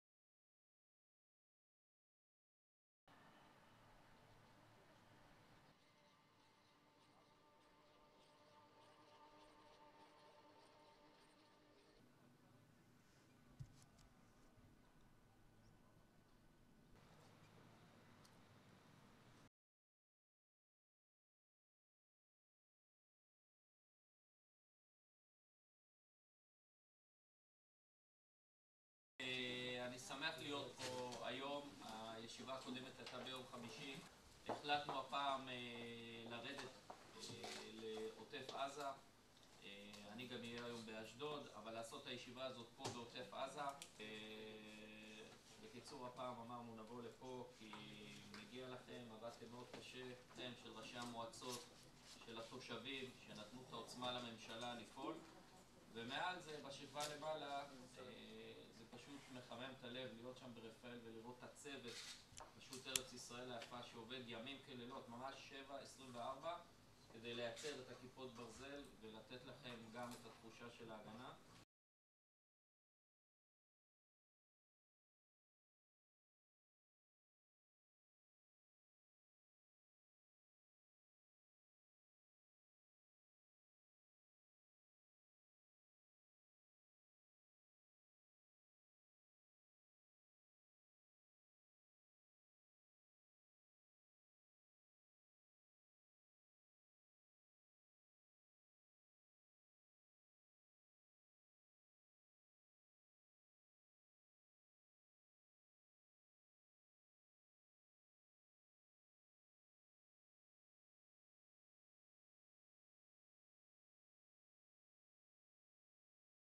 מנכ"ל משרד רה"מ הראל לוקר מסייר בעוטף עזה ובאשדוד
צילום: לע"מ, איתי בית און